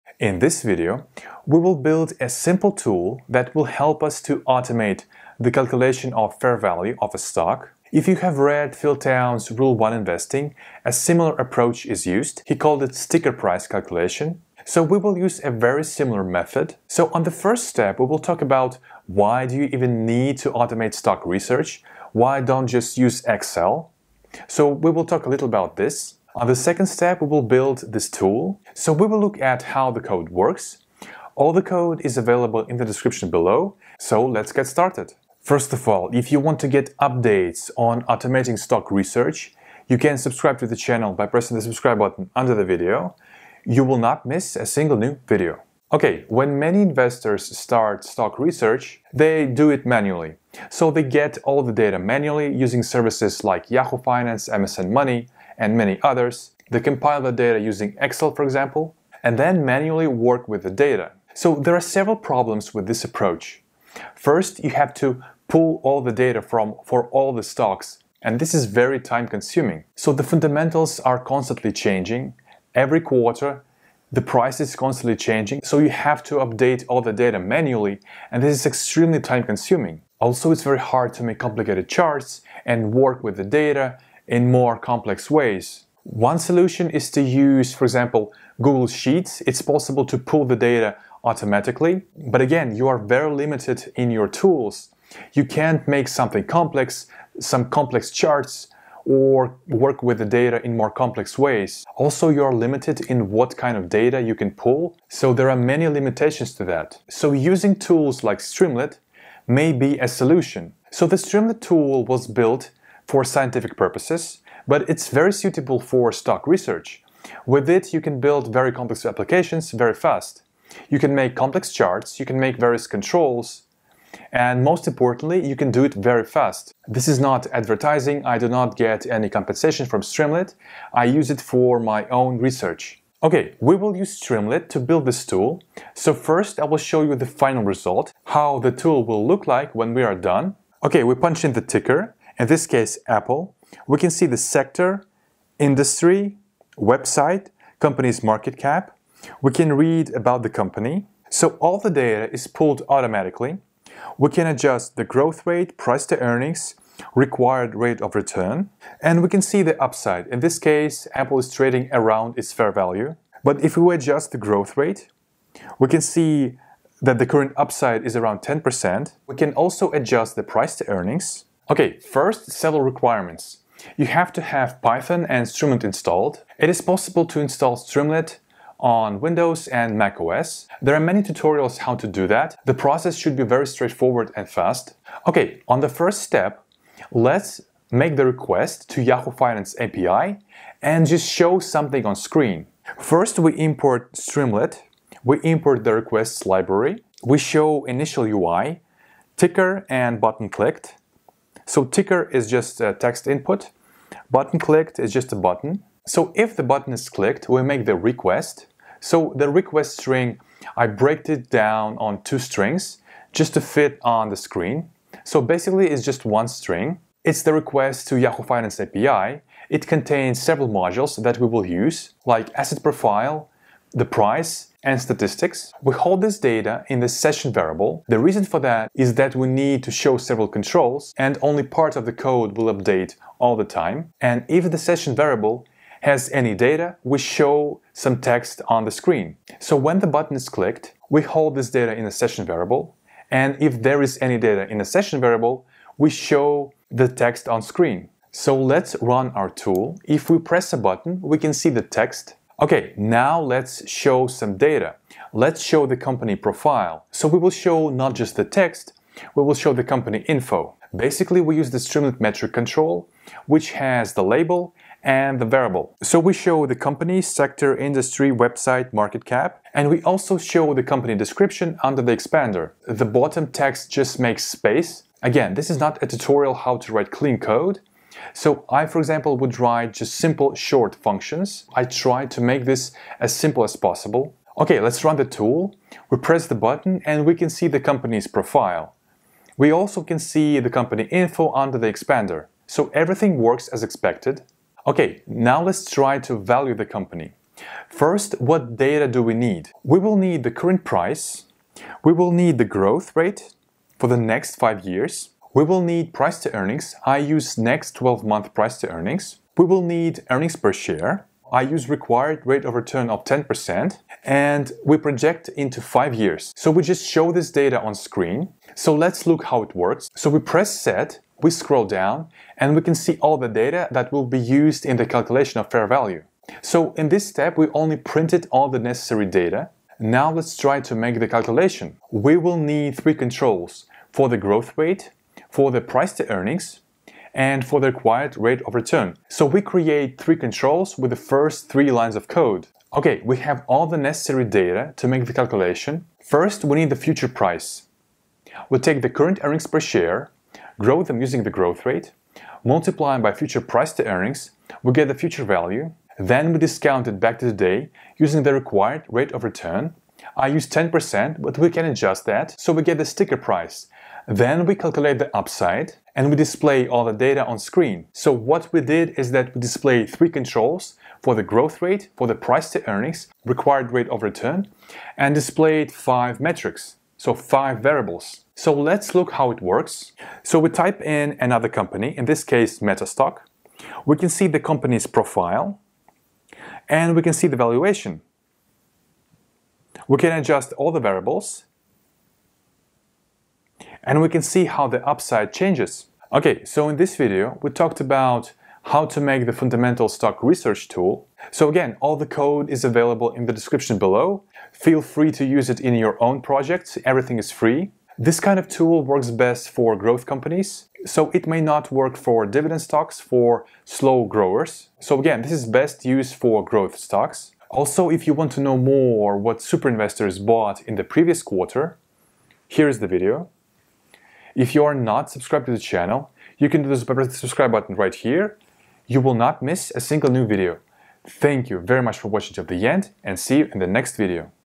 How to Calculate Stock Fair Value Automatically - Phil Town’s Rule #1 STICKER PRICE with Streamlit
# Code for the tutorial
import streamlit as st
import requests

def pv(fv,requiredRateOfReturn,years):
    return fv / ((1 + requiredRateOfReturn / 100) ** years)

def fv(pv,growth,years):
    return pv * (1 + growth)  ** years

# Initial UI
ticker = st.text_input('Ticker', "AAPL").upper()
buttonClicked = st.button('Set')

# Callbacks
if buttonClicked:
  modules = f"""modules=assetProfile%2Cprice%2CfinancialData%2CearningsTrend%2CdefaultKeyStatistics"""
  requestString = link + modules

  request = requests.get(f"{requestString}", headers={"USER-AGENT": "Mozilla/5.0"})
  json = request.json()
  data = json["quoteSummary"]["result"][0]

  st.session_state.data = data

if 'data' in st.session_state:
  # st.header("Hello World")

  data = st.session_state.data

  # Print company profile
  st.header("Company Profile")
  st.metric("Sector", data["assetProfile"]["sector"])
  st.metric("Industry", data["assetProfile"]["industry"])
  st.metric("Website", data["assetProfile"]["website"])
  st.metric("Market Cap", data["price"]["marketCap"]["fmt"])
  with st.expander("About Company"):
    st.write(data["assetProfile"]["longBusinessSummary"])

  # Add space between sections
  st.markdown("", unsafe_allow_html=True)
  st.markdown("", unsafe_allow_html=True)

  # Get the metrics needed for valuation
  st.header("Valuation")
  currentPrice = data["financialData"]["currentPrice"]["raw"]
  growth = data["earningsTrend"]["trend"][ 4 ][ "growth" ][ "raw" ] * 100
  peFWD = data["defaultKeyStatistics"]["forwardPE"]["raw"]
  epsFWD = data["defaultKeyStatistics"]["forwardEps"]["raw"]
  requiredRateOfReturn = 10.0
  yearsToProject = 5

  # # Print the metrics
  # st.metric("currentPrice", currentPrice)
  # st.metric("growth", growth)
  # st.metric("peFWD", peFWD)
  # st.metric("epsFWD", epsFWD)
  # st.metric("requiredRateOfReturn", requiredRateOfReturn)
  # st.metric("yearsToProject", yearsToProject)

  # Add controls
  growth = st.number_input("Growth", value=growth, step = 1.0)
  peFWD = st.number_input("P/E", value=peFWD, step = 1.0)
  requiredRateOfReturn = st.number_input("Required Rate Of Return", value=requiredRateOfReturn, step = 1.0)

  # Fair value calculation
  futureEPS = fv(epsFWD,growth/100,yearsToProject)
  futurePrice = futureEPS * peFWD
  stickerPrice = pv(futurePrice, requiredRateOfReturn, yearsToProject)
  upside = (stickerPrice - currentPrice)/stickerPrice * 100

  # Show result
  st.metric("EPS", "{:.2f}".format(futureEPS))
  st.metric("Future Price", "{:.2f}".format(futurePrice))
  st.metric("Sticker Price", "{:.2f}".format(stickerPrice))
  st.metric("Current Price", "{:.2f}".format(currentPrice))
  st.metric("Upside", "{:.2f}".format(upside))